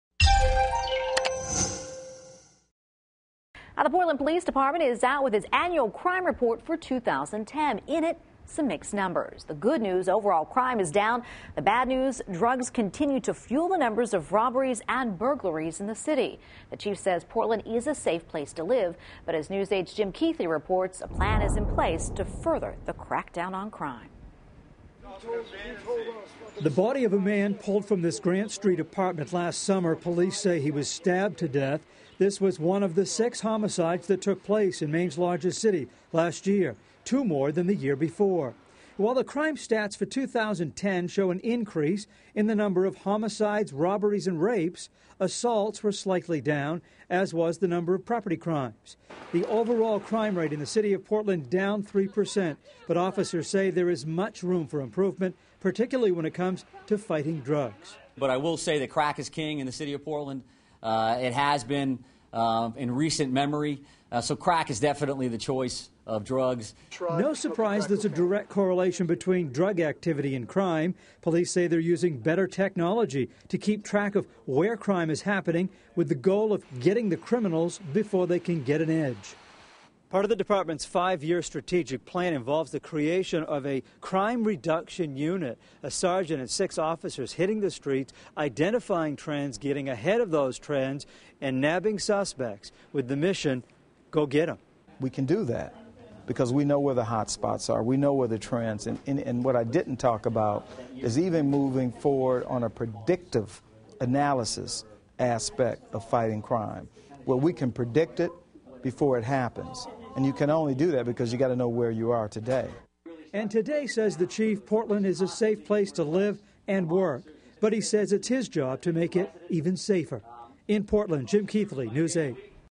Overall Crime Is Down According To Portland Police Crime Report
While overall crime is down according to the Portland Police Department Annual Crime Report, fighting drugs is still a priority. News 8's Jim Keithley reports.